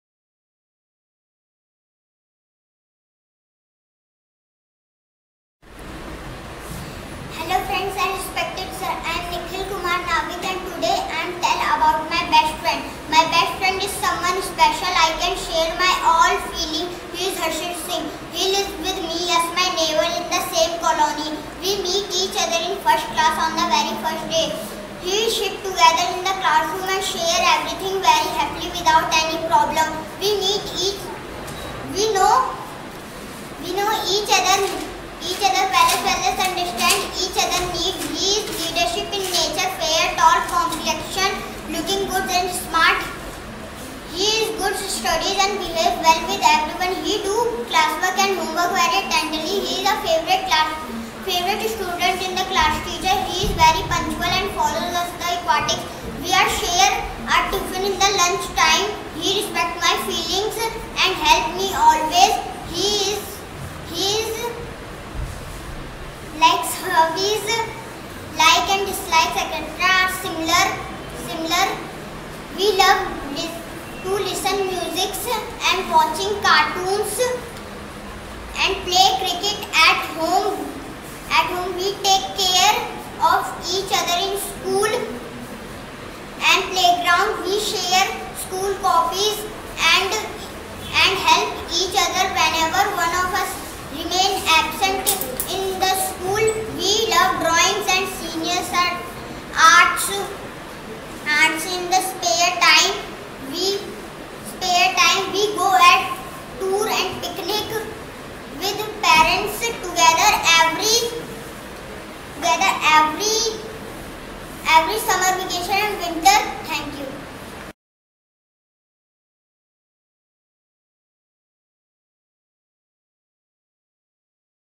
Nikhil Kumar delivers speech on his best friend taught by Rajan sharma
Neekhil kumar navik Delivers speech on his best friend taught by Rajan Sharma.
This video is made for only English Learner Students.